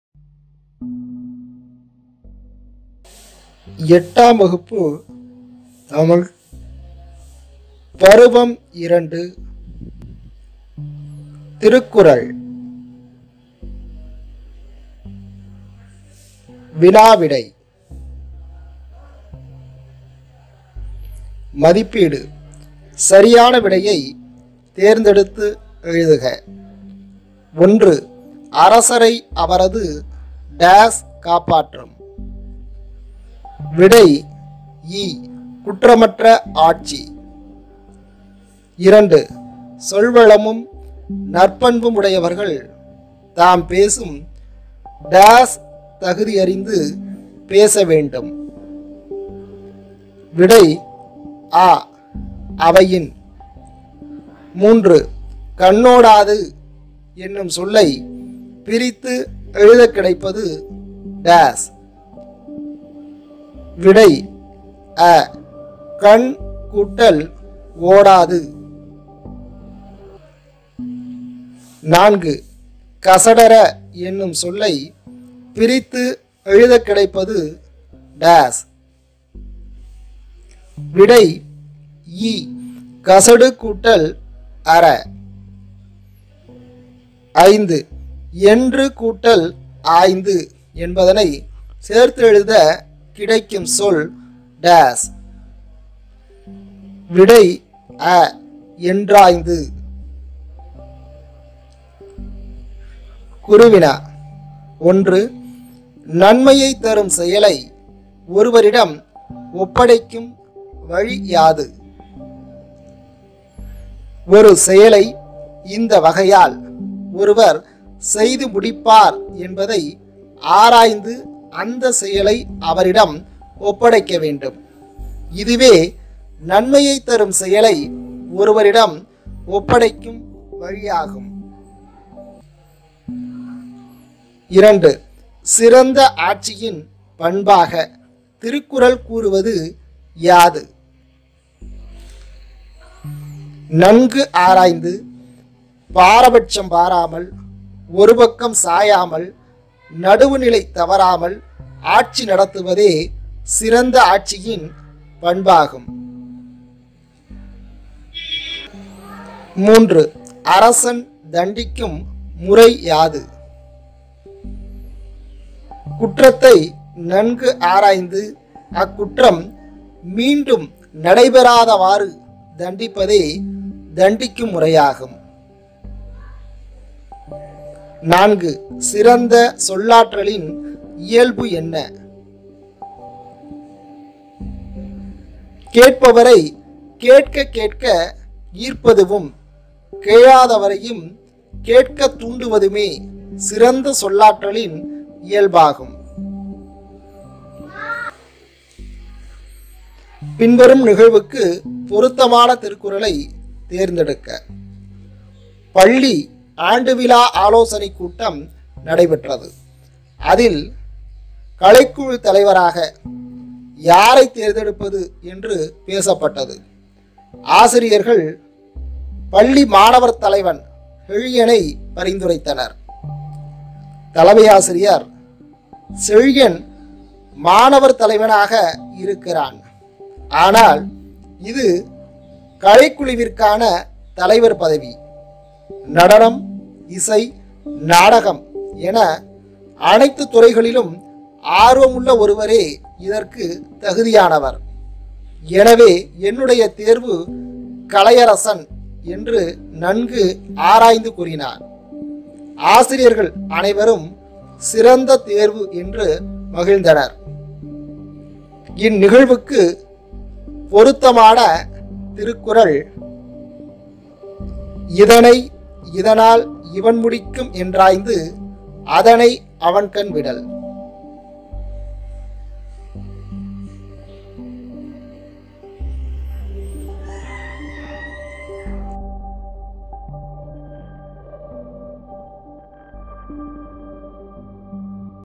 திருக்குறள் - 8வது தமிழ் - பருவம் 2 - வினா விடை
எட்டாம் வகுப்பு – தமிழ் – பருவம் 2 - திருக்குறள் எனும் பாடப் பகுதிக்கான மதிப்பீட்டிற்கான வினா – விடை காணொளி. பள்ளி மாணவர்களுக்கும், ஆசிரியர்களுக்கும் பயனுள்ள காணொளி @vikatabharathi9169